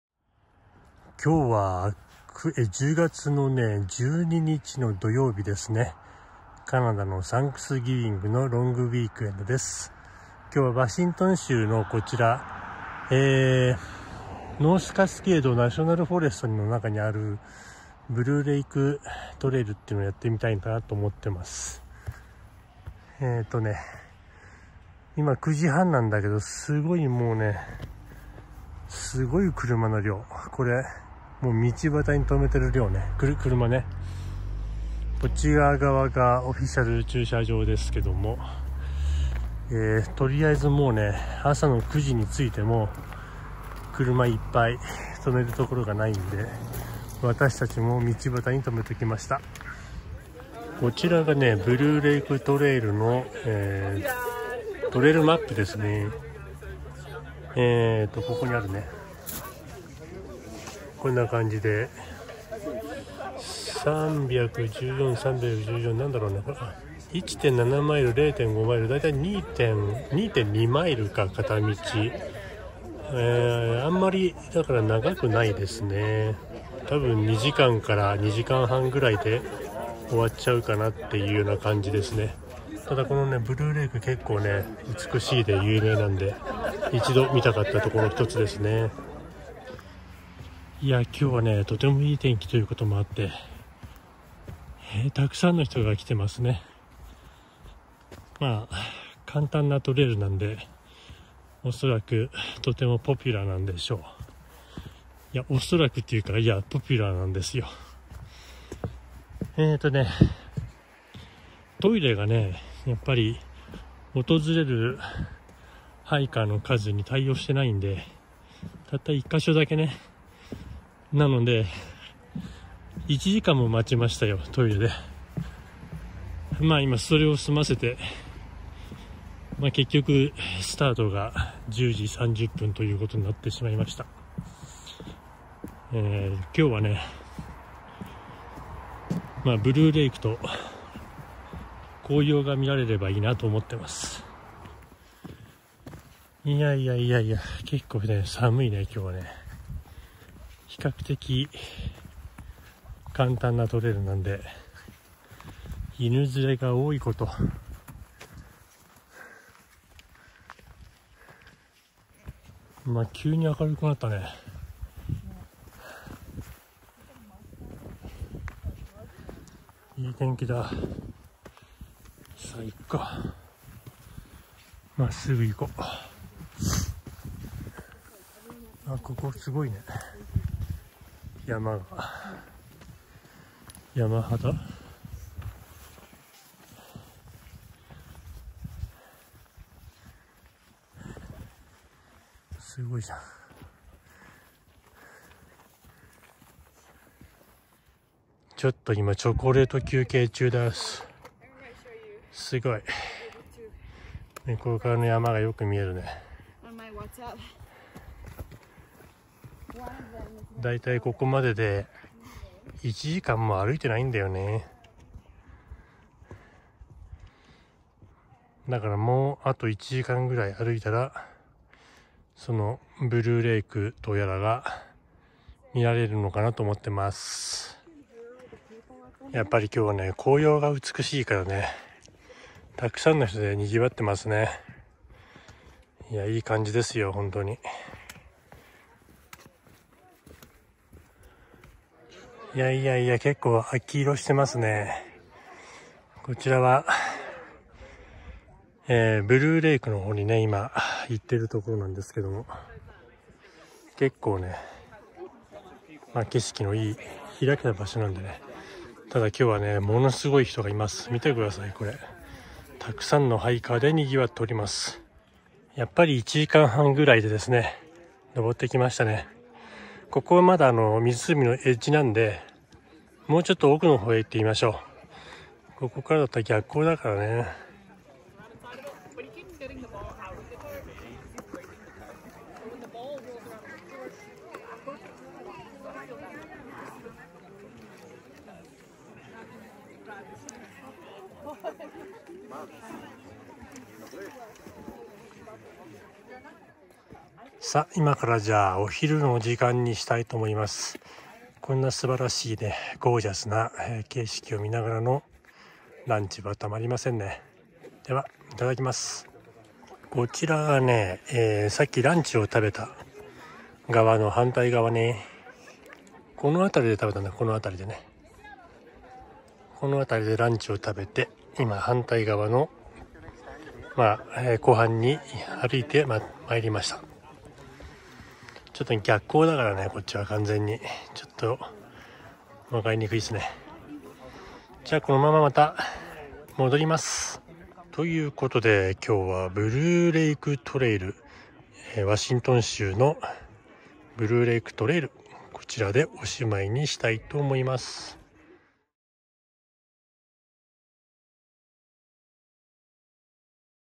[ハイキング] ワシントン州 Blue Lake Trail へ紅葉を見に行こう
１０月の晴れた週末。紅葉のBlue Lakeを見に行こうと思いました。ワシントン州の国立公園内にある比較的簡単なトレイル。朝９時に到着するも、すでに駐車場は一杯。１マイルも先のハイウェイ上に路肩駐車。トレイルはもちろん大混雑。そしてBlue Lakeの湖畔も大賑わい。でもみんな綺麗なもの見たいからね。大変な思いして登ってきて、美しい景色を見られるのはその報酬です。